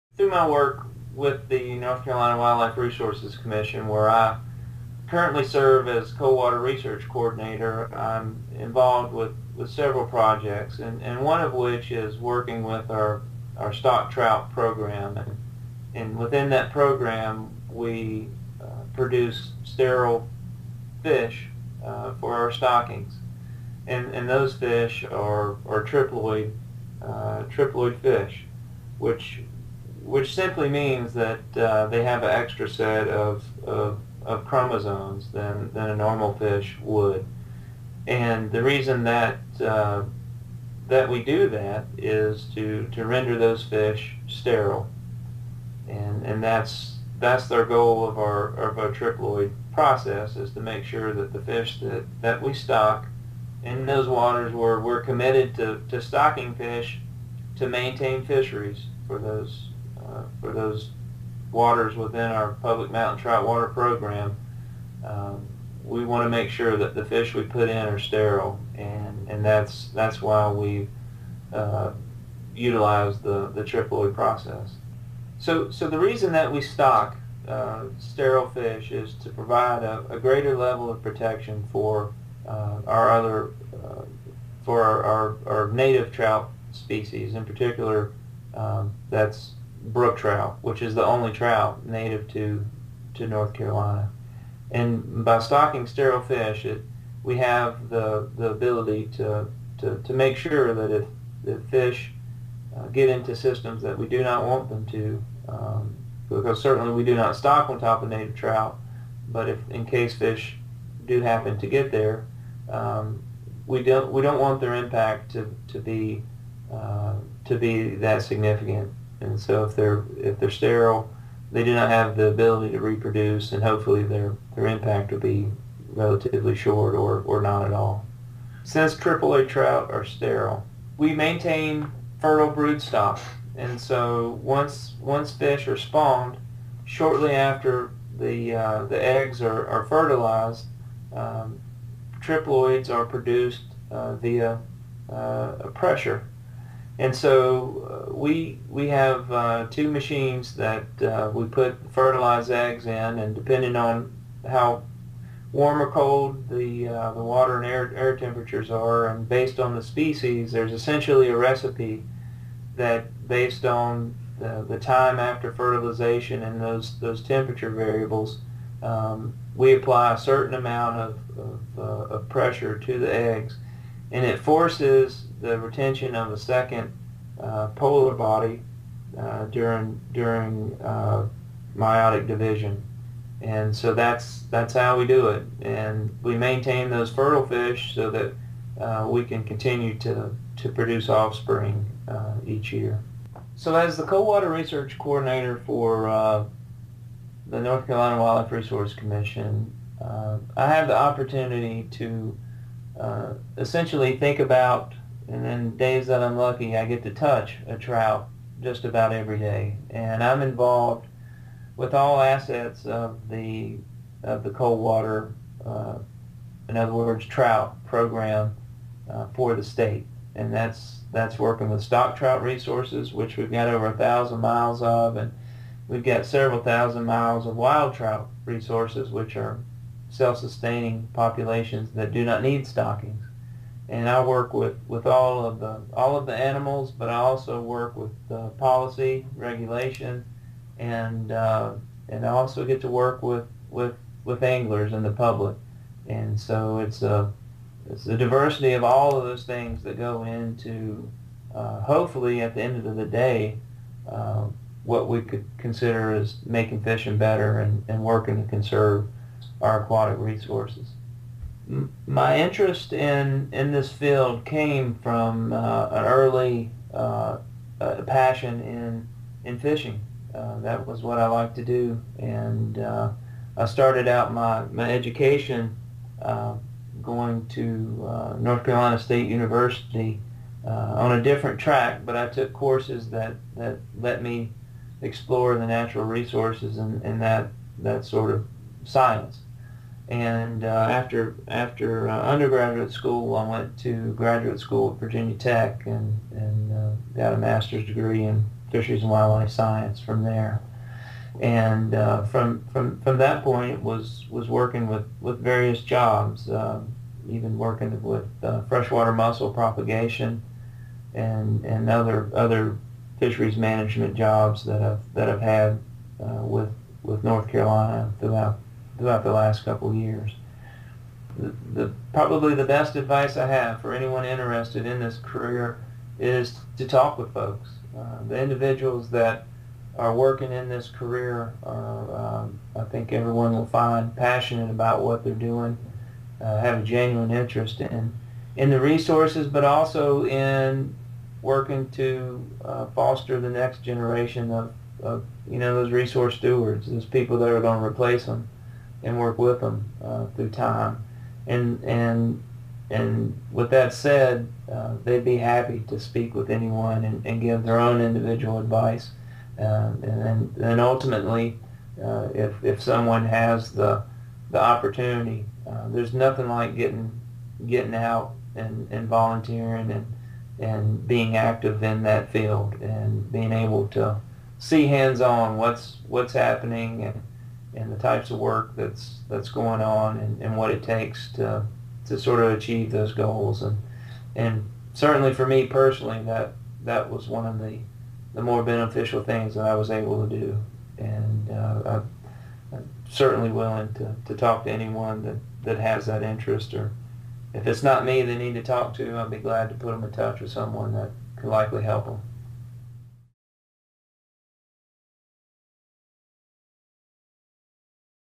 NC Wildlife: How and Why Sterile Trout are Stocked in NC, Becoming a Fish Biologist
This video provides an overview of how and why the Commission produces these sterile fish. In addition, there is discussion about the field of fisheries science and how folks can become involved.

N.C. Wildlife Resources Commission Coldwater Research Coordinator Jacob Rash speaks with Christine Muth, a biology teacher with the N.C. School of Science and Math, to discuss stocking sterile (triploid) trout into North Carolina's mountain trout waters. They also discuss careers in fisheries.

Visit the Wildlife Commission's trout fishing page to learn more about trout resources, angling maps, regulations, and much more

Video courtesy of Christine Muth and Rob Caldwell, N.C. School of Science and Math